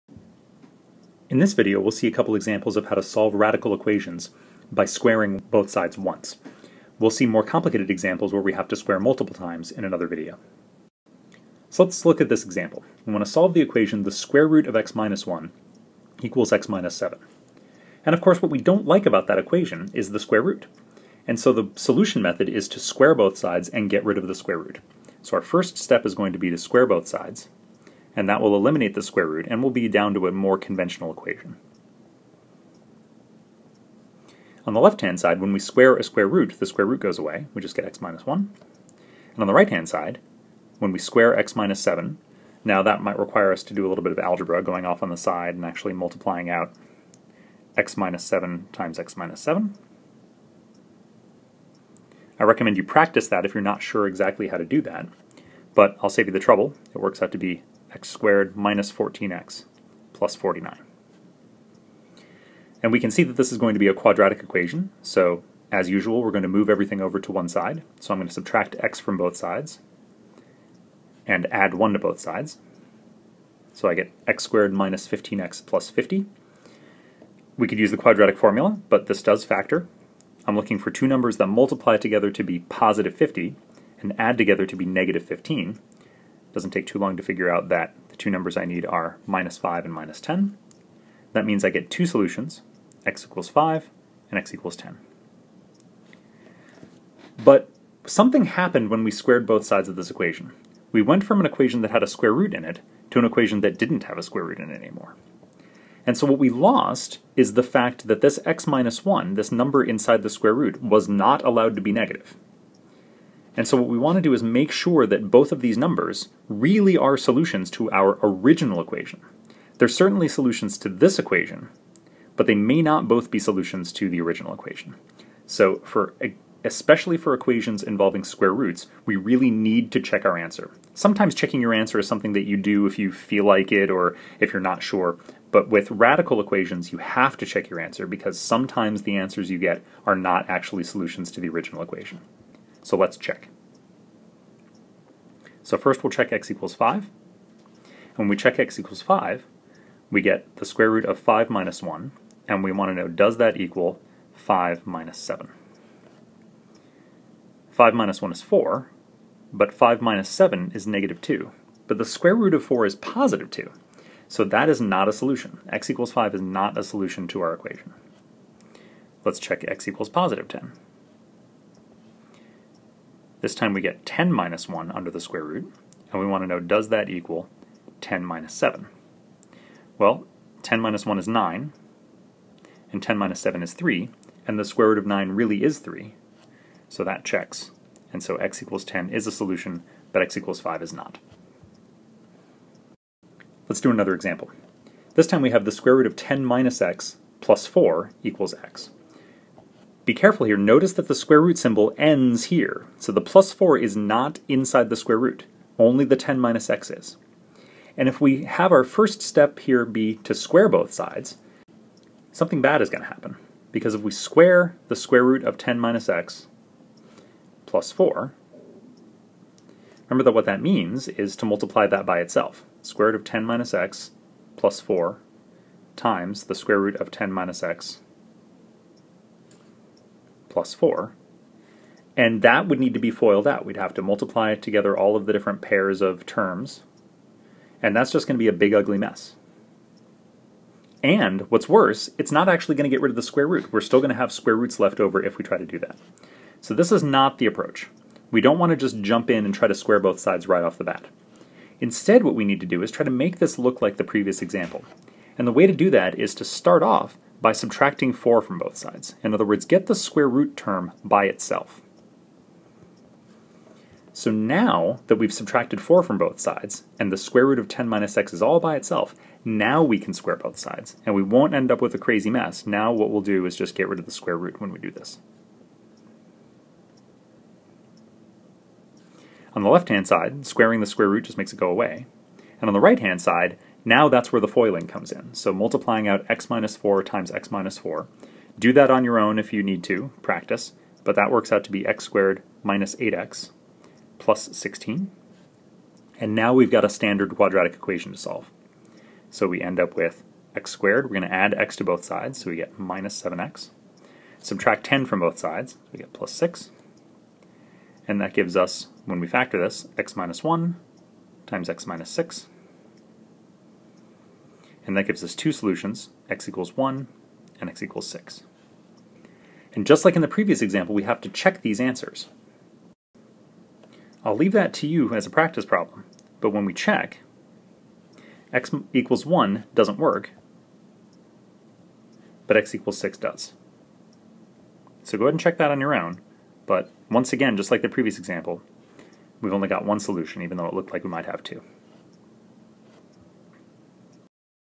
Example: Solving a Radical Equation (Square Once)
In this video, we see some examples of solving radical equations where we must square both sides once to get rid of the radical sign.